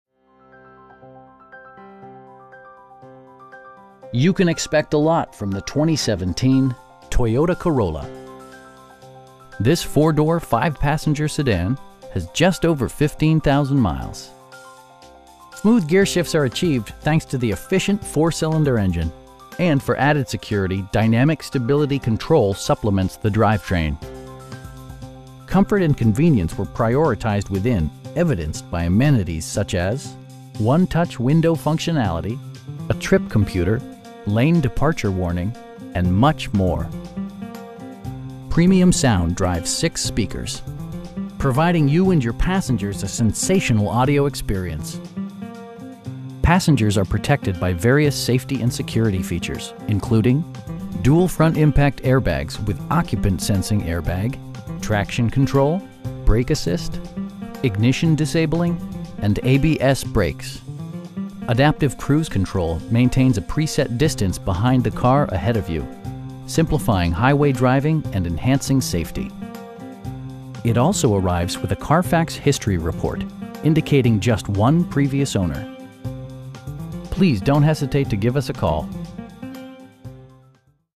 2017 Toyota Corolla SE in Marion, IL 62959
Marion Toyota
3300 W Deyoung

You can expect a lot from the 2017 Toyota Corolla. This 4 door, 5 passenger sedan has just over 15,000 miles! Smooth gearshifts are achieved thanks to the efficient 4 cylinder engine, and for added security, dynamic Stability Control supplements the drivetrain. Comfort and convenience were prioritized within, evidenced by amenities such as: 1-touch window functionality, a trip computer, air conditioning, tilt and telescoping steering wheel, power door mirrors, lane departure warning, and much more. Premium sound drives 6 speakers, providing you and your passengers a sensational audio experience. Passengers are protected by various safety and security features, including: dual front impact airbags with occupant sensing airbag, head curtain airbags, traction control, brake assist, ignition disabling, and ABS brakes. Adaptive cruise control maintains a pre set distance behind the car ahead of you, simplifying highway driving and enhancing safety. It also arrives with a Carfax history report, indicating just one previous owner. We know that you have high expectations, and we enjoy the challenge of meeting and exceeding them! Please don't hesitate to give us a call.